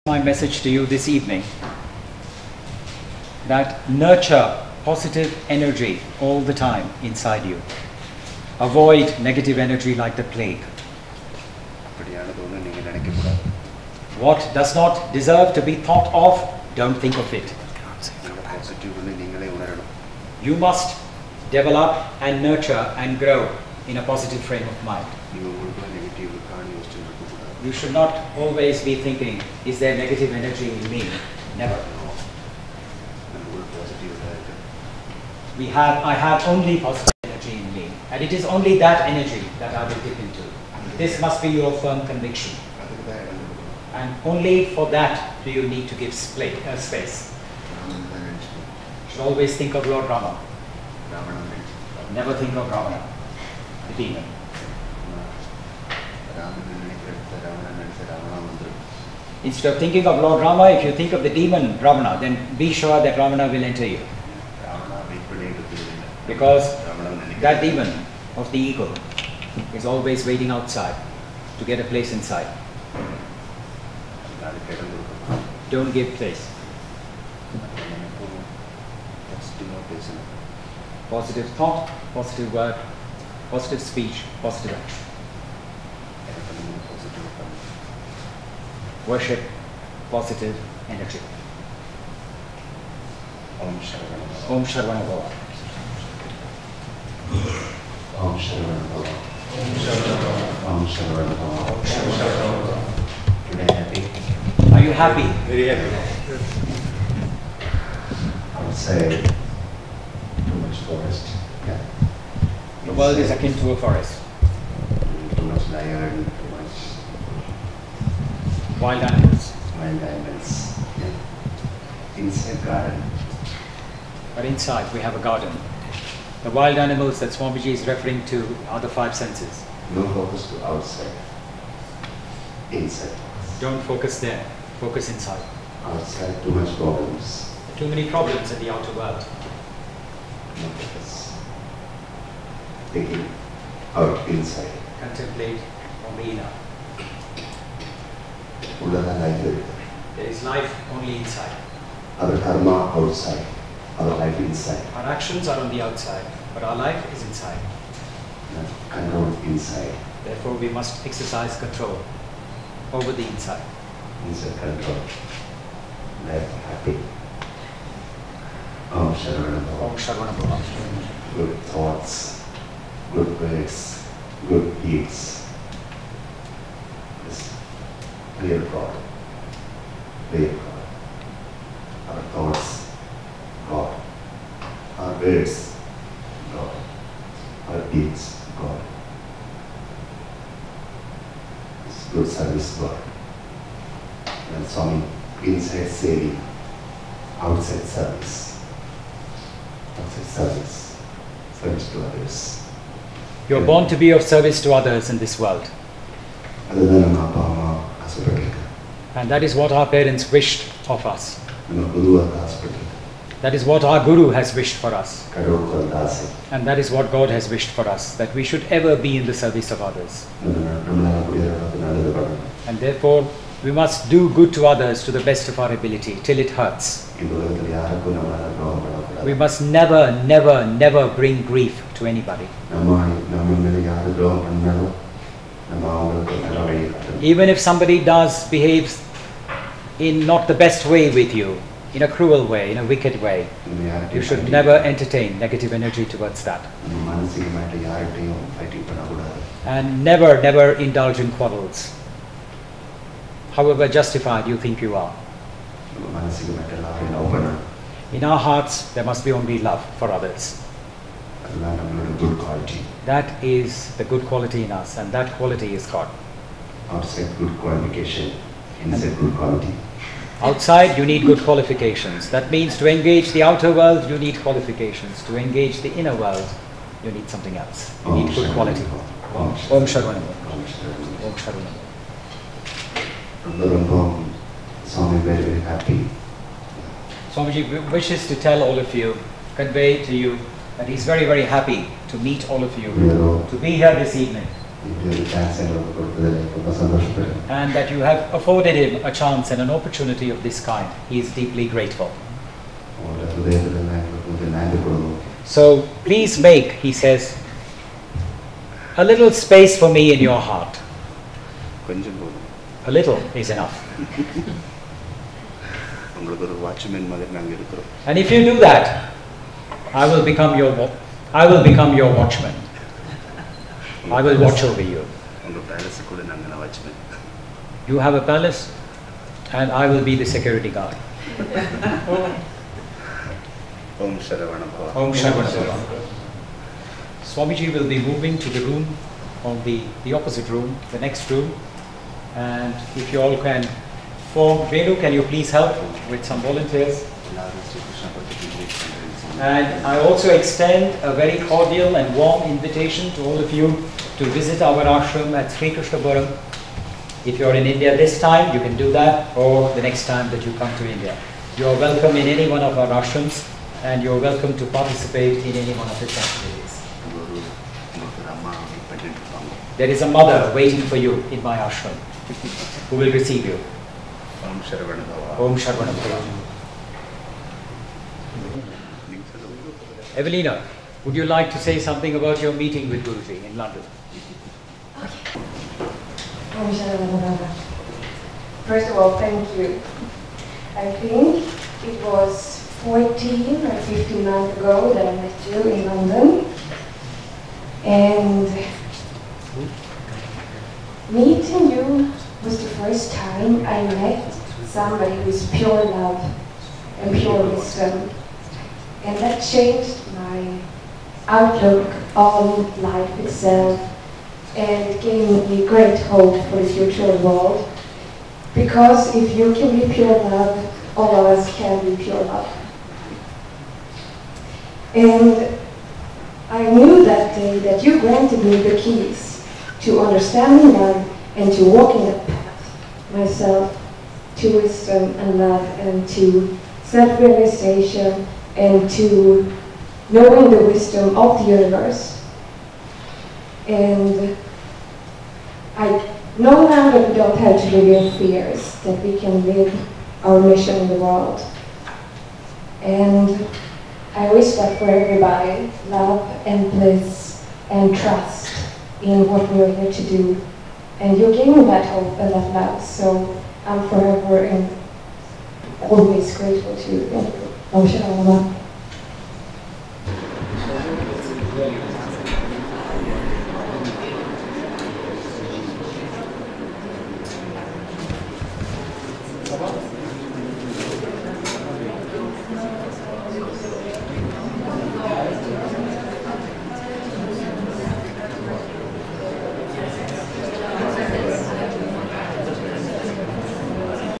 Sadguru Sri Sharavana Baba at IIMB Part 3 of 3
Sadguru Sri Sharavana Baba speaking at the International Spirituality Conference held at the Indian Institute of Management, Bangalore. January 2012.